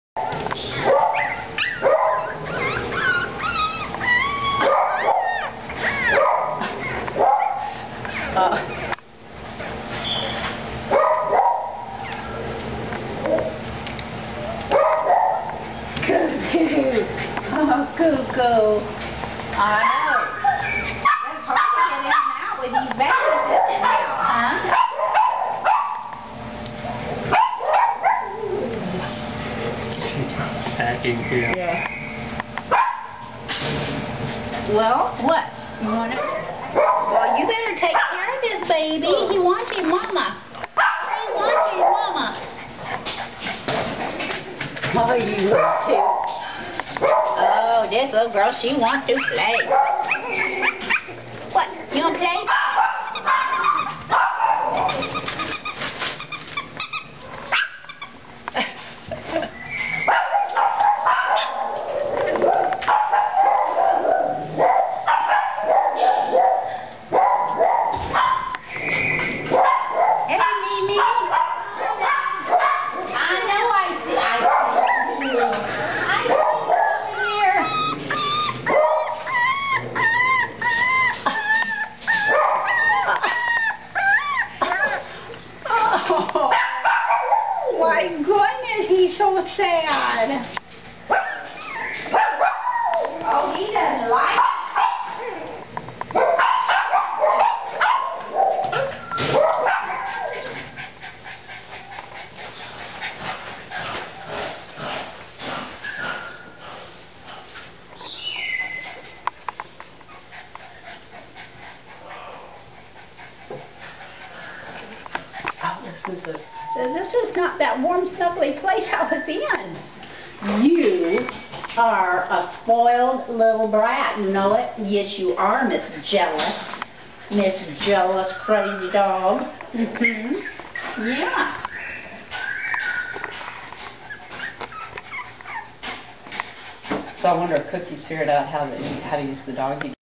Cookie's puppies 5-9-07
They were moved to the puppy pen. The big boy was a little upset about it.